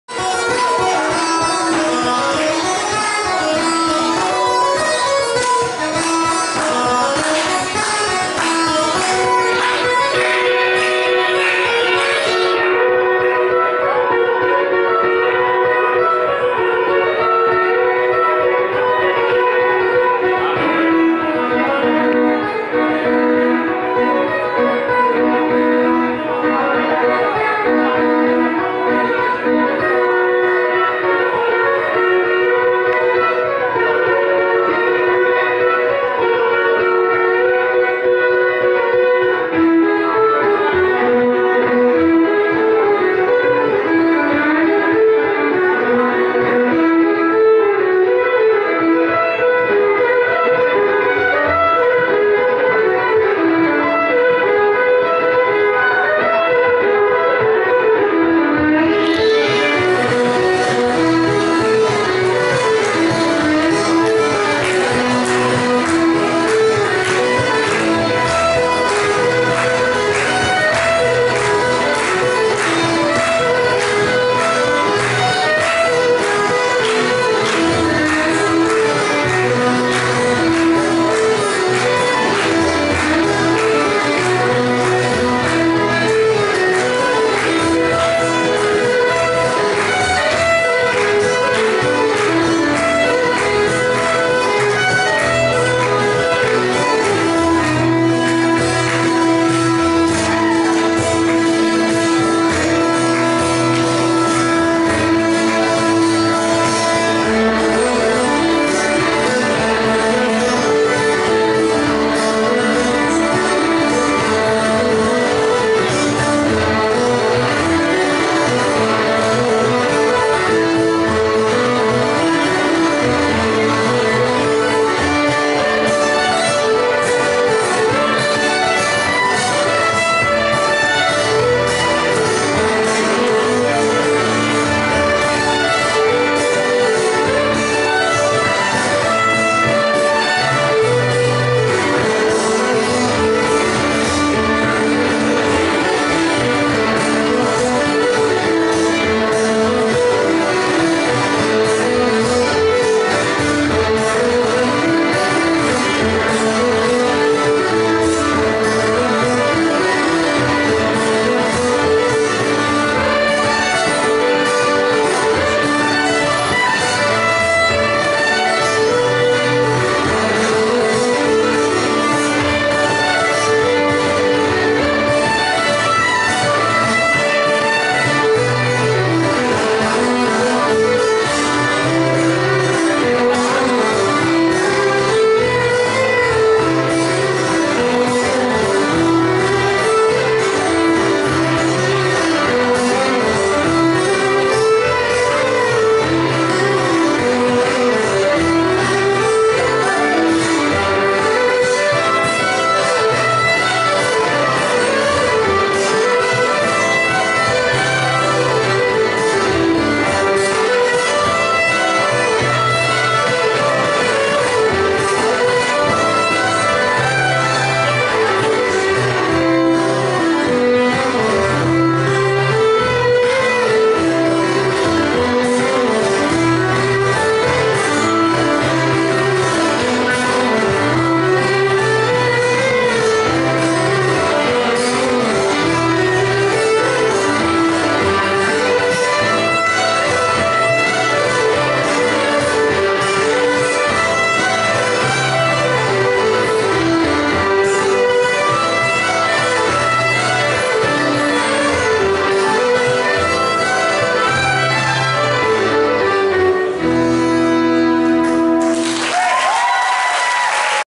Frogwater
Frogwater at Milwaukee's 2013 Irish Fest on Sunday, August 18, 2013. My apologies for the shakiness of this video. Only one video of them since I actually got to this stage early for the next performer. Wish I'd gotten there earlier.